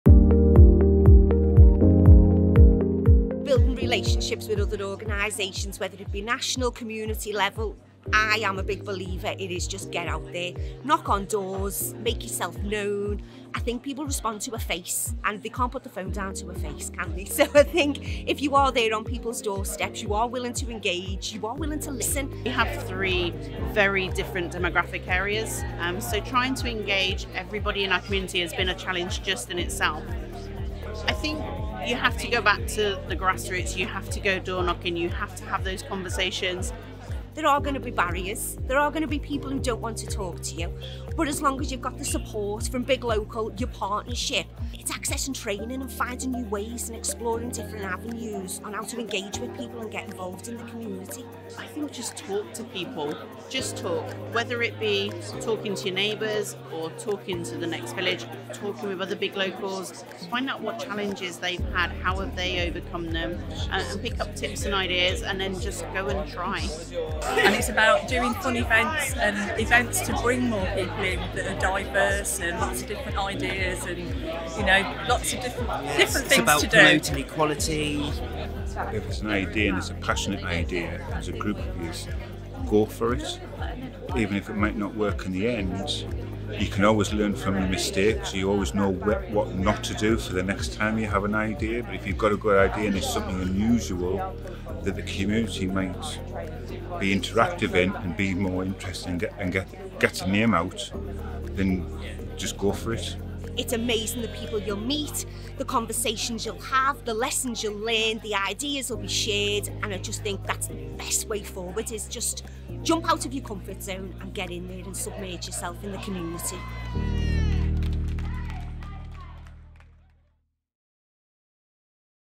How do you connect with your community?
At our annual Big Local networking event - Connects - we asked partnership members to share some of the ways they connect with their communities and their top tips.